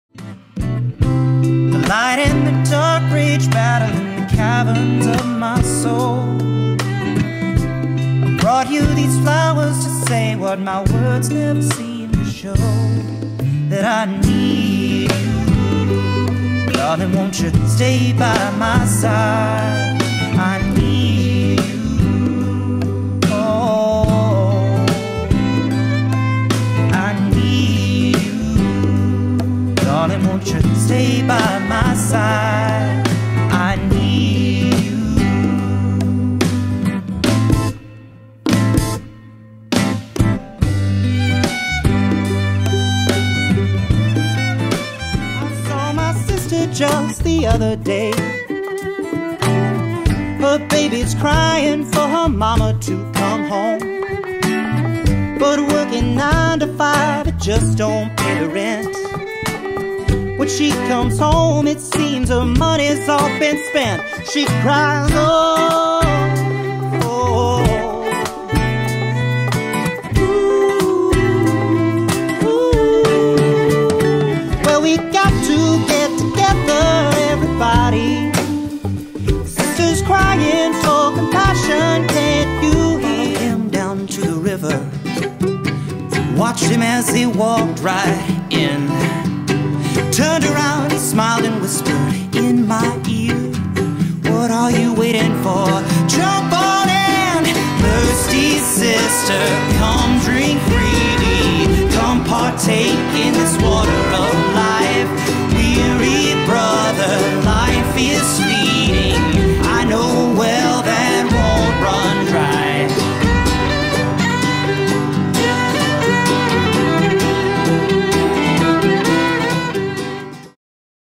Matthew Bell and the Next of Kin
Take a look at our first sound stage production and filming, featuring the unique folk soul sound of Matthew Bell and the Next of Kin…